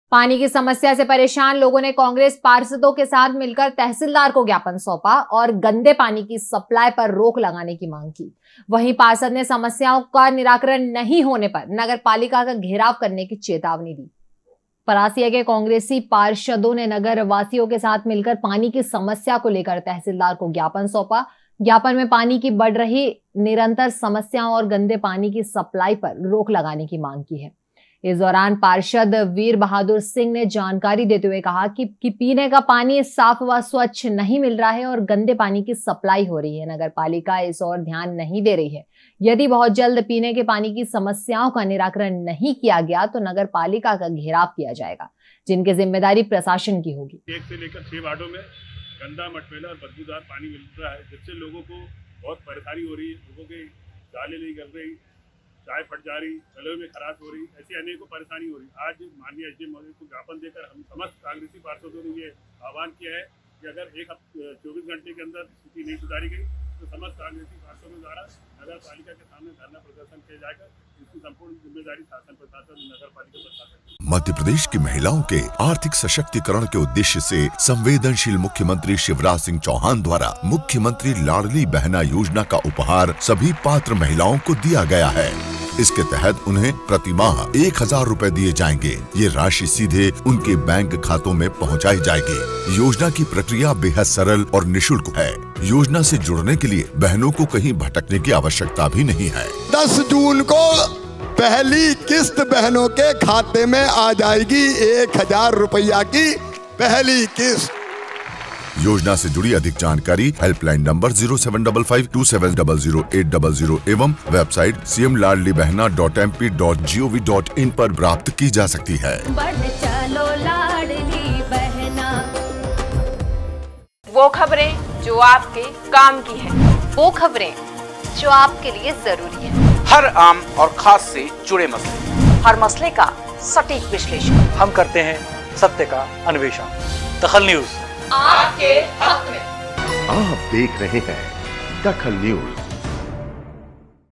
DAKHAL NEWS कांग्रेसी पार्षदों ने उठाई पानी की समस्या
कांग्रेसी पार्षदों ने उठाई पानी की समस्या
पानी के लिए तहसीलदार को ज्ञापन सौंपा